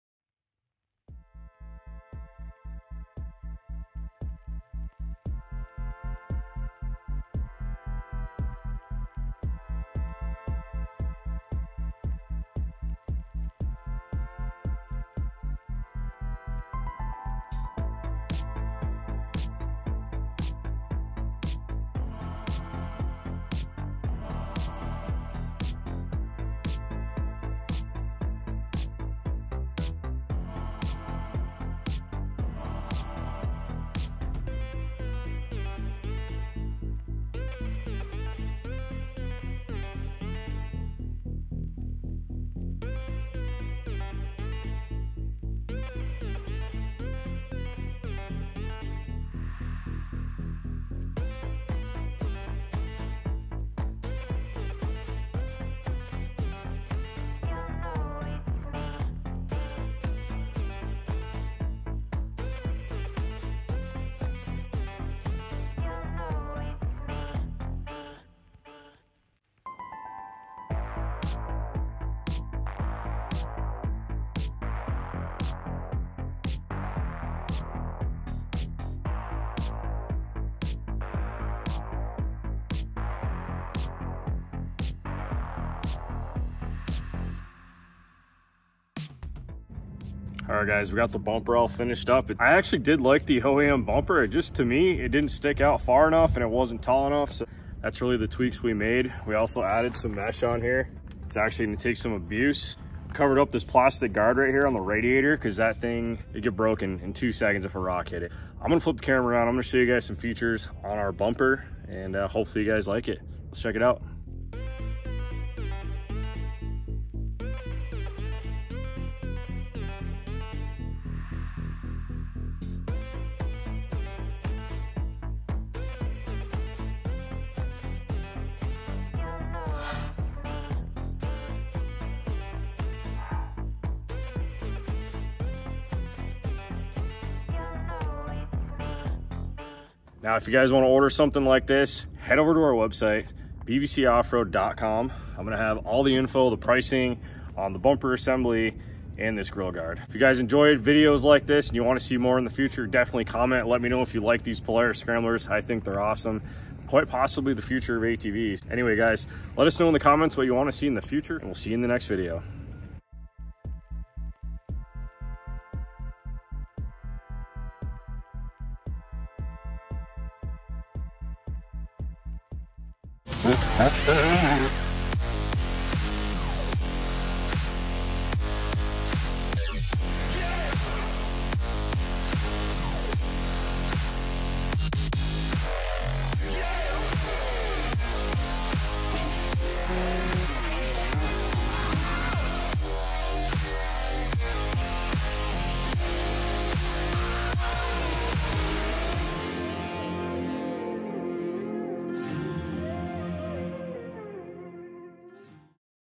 ⚡️Bumper Design & Build - Polaris SCRAMBLER XP1000S
In this video we design and fabricate a new bumper and grille for a 2023 polaris scrambler xp1000s ATV.

Follow along as i show 3d scanning , cad design, tig welding to finished product.